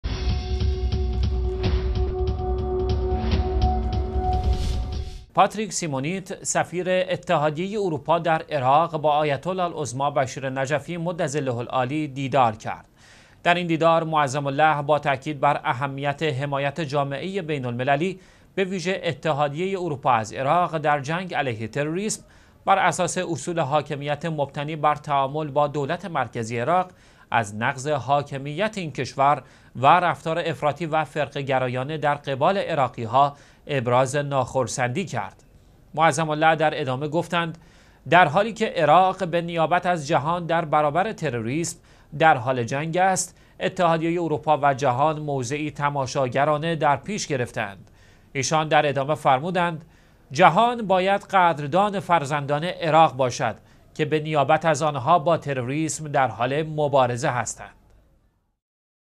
ديدار نماینده اتحادیه اروپا در عراق، با آیت الله العظمى بشیر نجفی
«پاتریک سیمونیت»، سفیر اتحادیه اروپا در عراق، با آیت الله العظمی بشیر نجفی (مدظله العالی) دیدار کرد.
در این دیدار، معظم له با تاکید بر اهمیت حمایت جامعه بین المللی به ویژه اتحایه اروپا از عراق؛ در جنگ علیه تروریسم، براساس اصول حاکمیت مبتنی بر تعامل با دولت مرکزی عراق؛ از نقض حاکمیت این کشور و رفتار افراطی و فرقه گرایانه در قبال عراقی ها، ابراز ناخرسندی کرد.
معظم له در ادامه گفتند: در حالی که عراق به نیابت از جهان در برابر تروریسم، در حال جنگ است، اتحادیه اروپا و جهان، موضعی تماشاگرانه در پیش گرفته اند.
ایشان در ادامه فرمودند: جهان باید قدردان فرزندان عراق باشد که به نیابت از آن ها با تروریسم در حال مبارزه هستند.

 دوشنبه - ۲ فروردین ۱۳۹۵
الإثنين - ١١ جمادي الثانيه ١٤٣٧